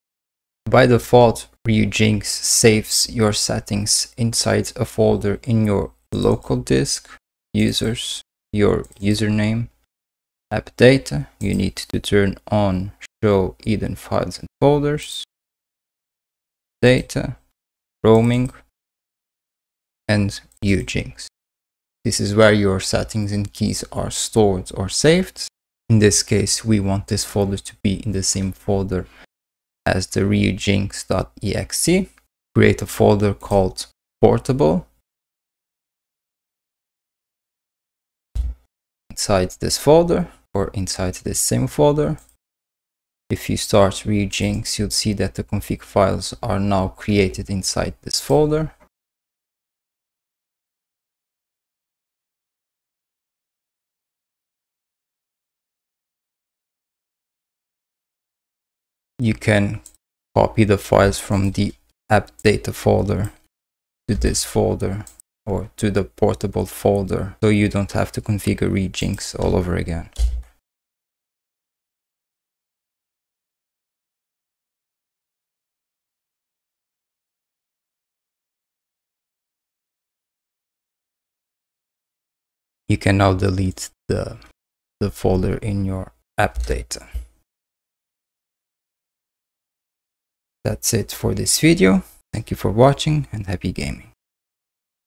ryujinx portable mode
Create a folder name "portable" inside your Ryujinx folder
Copy the config files from C:\Users\yourusername\AppData\Roaming\Ryujinx
and place them inside the portable folder.

PC Specs:
CPU: Intel(R) Core(TM) i5-10400F CPU @ 2.90GHz
RAM: 16GB
GPU: NVIDIA GeForce GTX 1660 SUPER